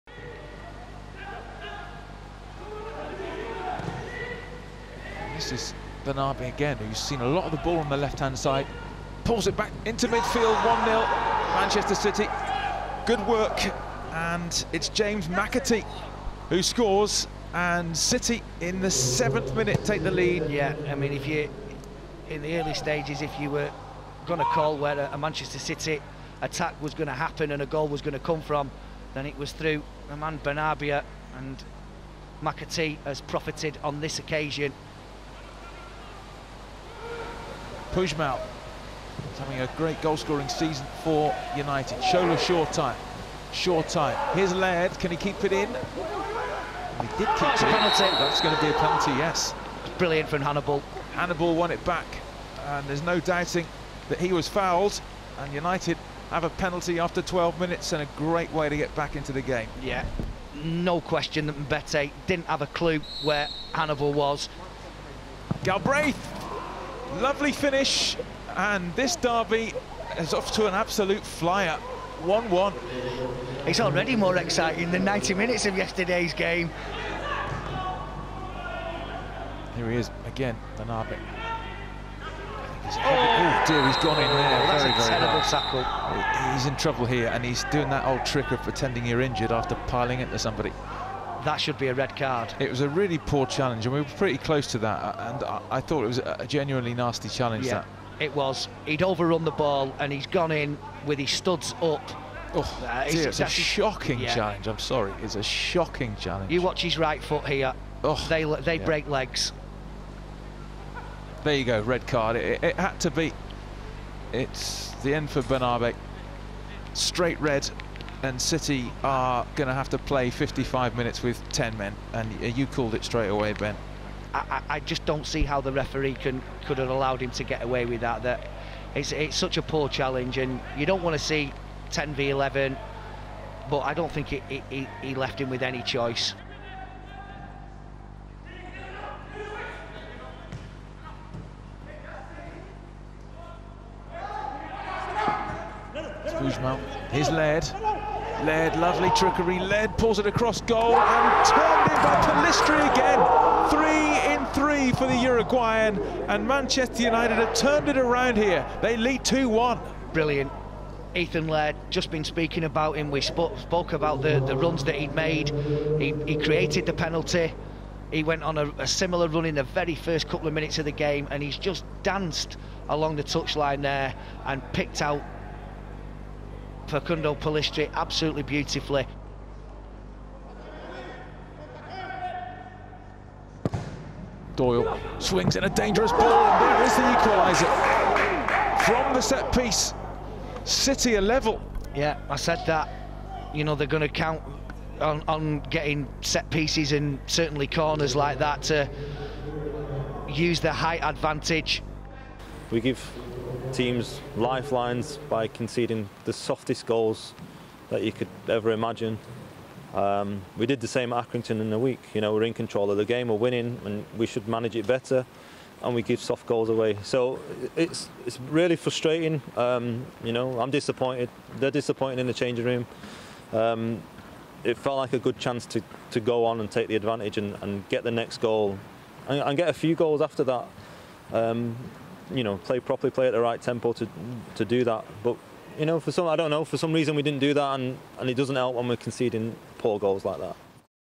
U23 Highlights | Manchester United 2-2 Manchester City | The Academy
See all the highlights as Manchester United and Manchester City deservedly shared the points after drawing 2-2 at Leigh Sports Village Stadium, with Ethan Galbraith and Facundo Pellistri on the scoresheet for the Reds.